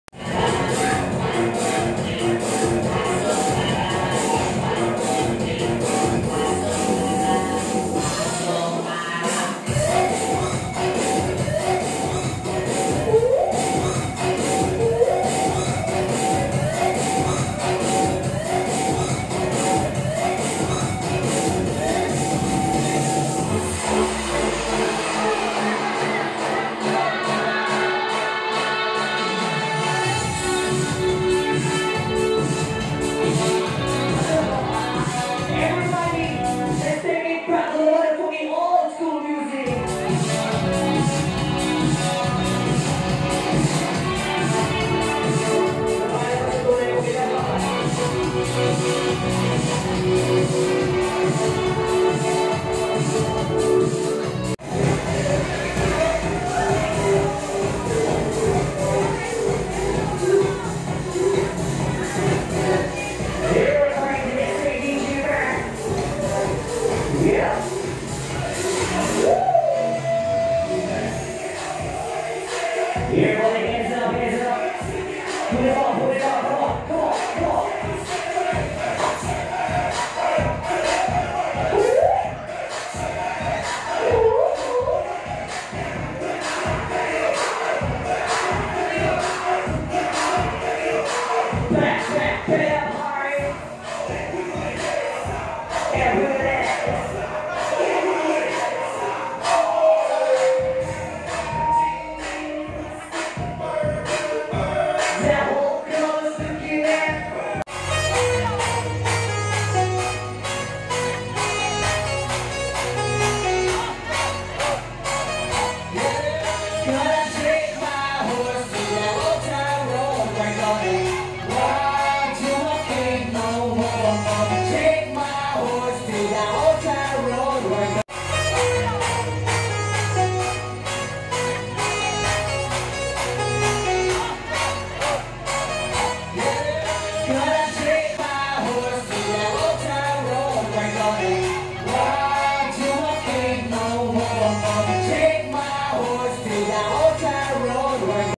DISCO @PadisPoint TIMOG💖
Together with the TEAM PARES BRANCH TIMOG AFTER CHRISTMASS PARTY Its time for them to have fun and enjoying there self😊